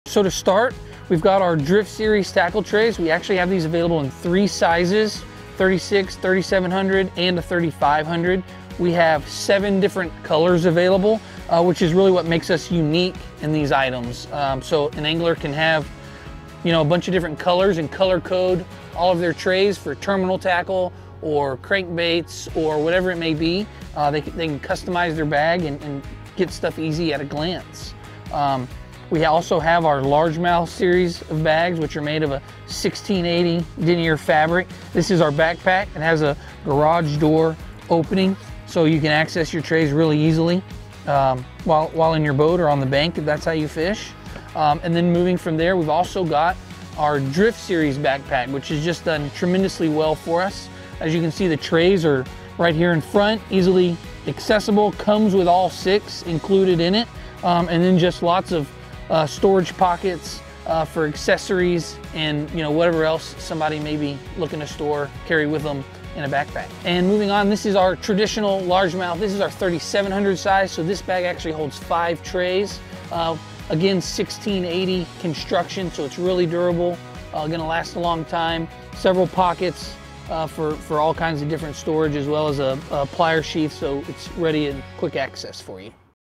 Evolution Fishing Offers Tackle Storage Solutions for All Anglers
Evolution Fishing was the presenting sponsor of the Pickwick Slam presented by Evolution Fishing.  Colt Fadal was on-site on behalf of Evolution Fishing to attend the event and support anglers.  Watch as Colt runs us through a variety of items from Evolution Fishing's lineup of products.